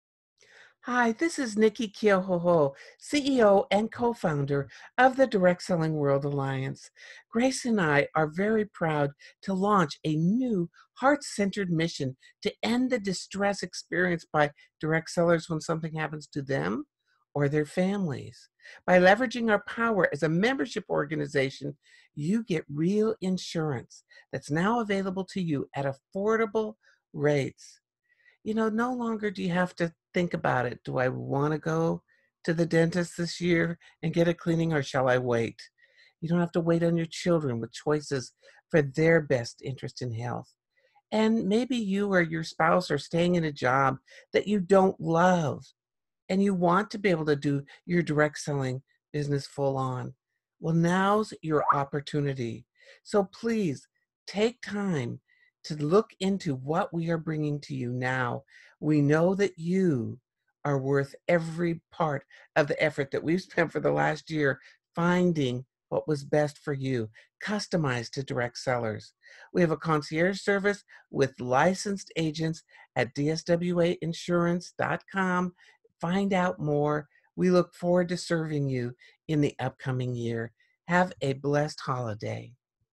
How to Protect Your Family with DSWA Insurance - Free Webinar with Nicki Keohohou
Thursday, December 12th at 3pm Eastern

The DSWA is proud to launch a new heart-centered mission to end the distress experienced by Direct Sellers when something happens to them or their family. By leveraging our power as a Membership organization, real insurance is now available for Direct Sellers at extremely affordable rates.

• No longer do you or your kids have to skip that exam, dental work or vision tests.

• No longer are you or your spouse tied to that job you're staying at for the benefits.

• No longer do you have to worry about your income evaporating if you pass away.

In this free Webinar, we'll cover unique risks to Direct Sellers, and how to protect you and your family if something happens to you or them. DSWA CEO & Co-Founder Nicki Keohohou will host the CEO of our USHealth1 partner, Bill Coleman, who have worked together tirelessly for nearly a year to establish these new options for Direct Sellers.

When you're ready, our DSWA Benefit Advisors are available by phone to craft a custom plan for you that fits your budget AND protects you and your family. These are licensed professionals who've received additional training from Nicki Keohohou to better serve the unique needs of Direct Sellers.